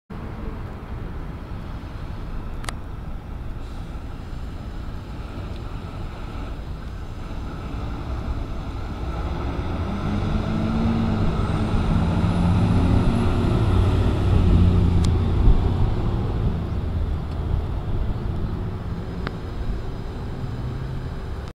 SCDF PL122
Singapore Civil Defence Force
Alexandra Fire Station ( Stn12 ) turnout: PL122
*Taken at time when pumper was Spare appliance*